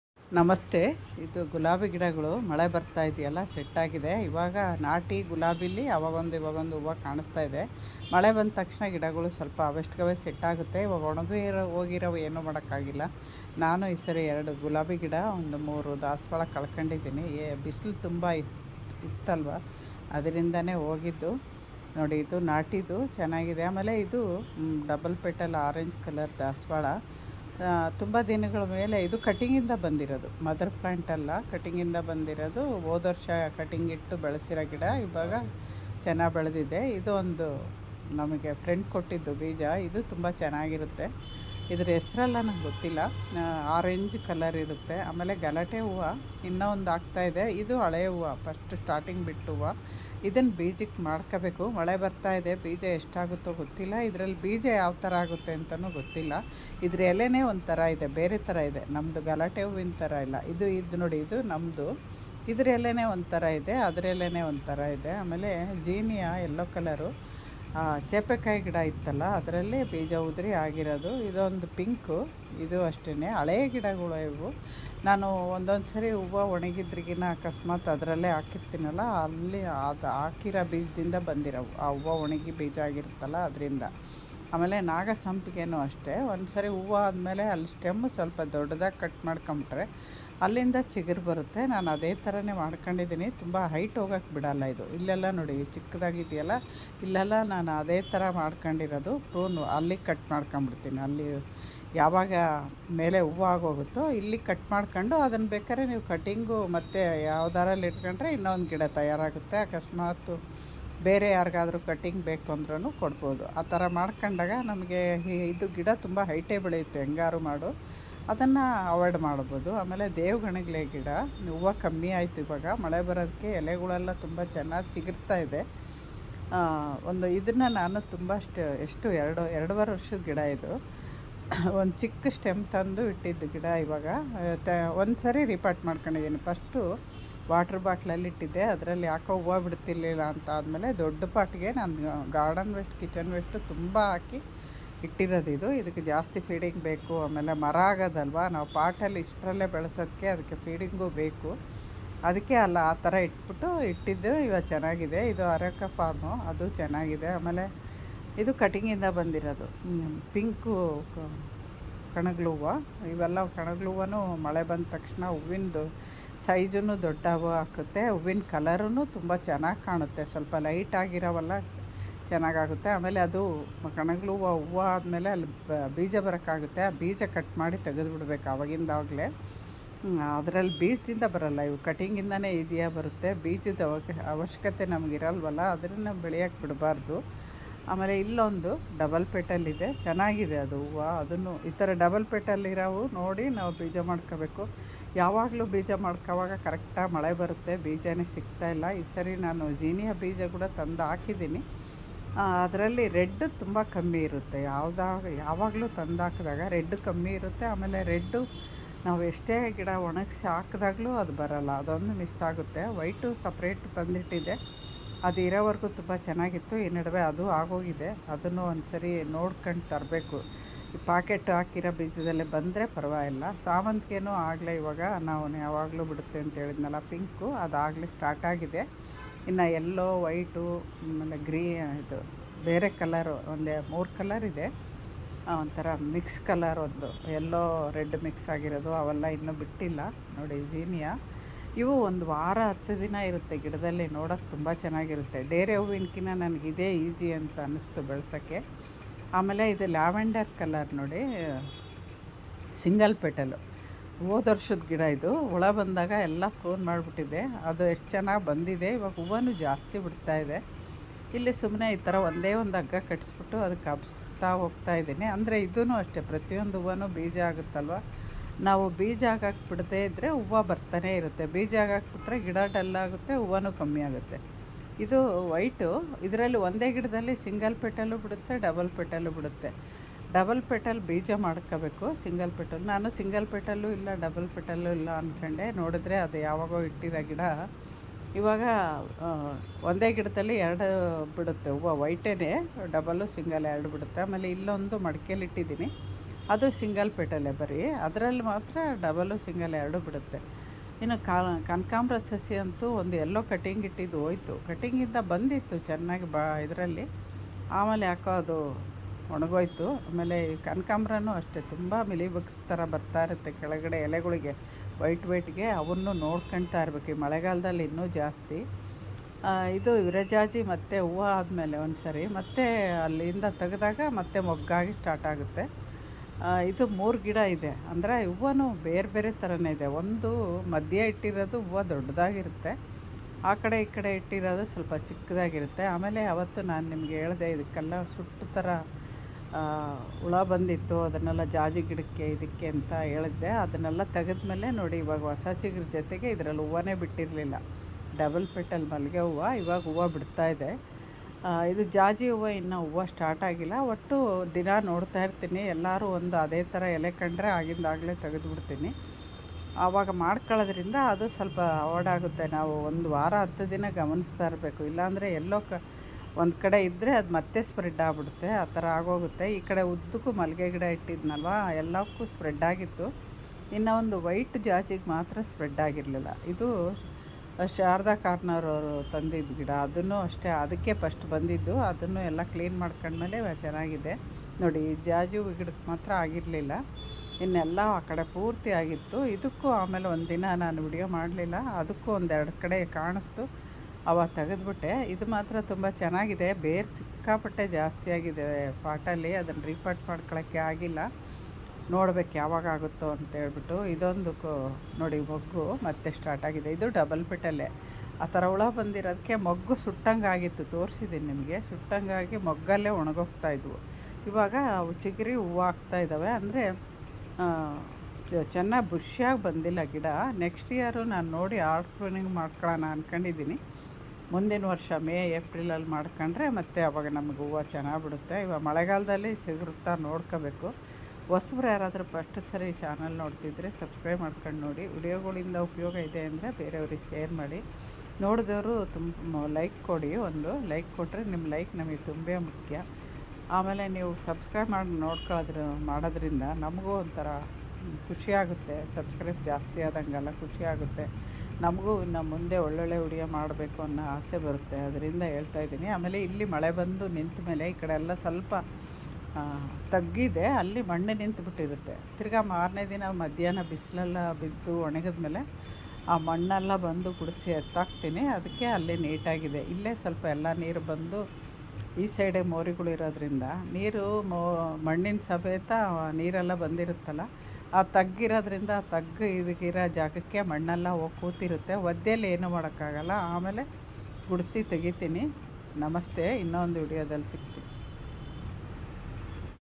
ಗಾರ್ಡನಲ್ಲಿ_ಇರುವ_ಹೊವುಗಳ_ಚೆಲವು_ನೋಡಿ//
ನಮ್ಮಸ್ಥೆ ಇಂದಿನ ವಿಡಿಯೋದಲ್ಲಿ ಗಾರ್ಡನ್ ಅಪ್ಡೇಟ್ ಹೂವುಗಳನ್ನು ತೋರಿಸುತ್ತಾ ಕೆಲವು ವಿಷಯಗಳು ಟೀಪ್ಸ ನಿಮ್ಮೊಂದಿಗೆ ಹಂಚಿಕೊಳ್ಳಬೇಕೆಂದಿದ್ದೇನೆ ಇಷ್ಟವಾದರೆ ಸಬ್ಸ್ಕಬ್ ಮಾಡಿ ಸರ್ಪೂಟ್ ಮಾಡಿ ಹಾಗೇ ಲೈಕ್ ಮಾಡಿ ಸಹಕರಿಸಿ//🙏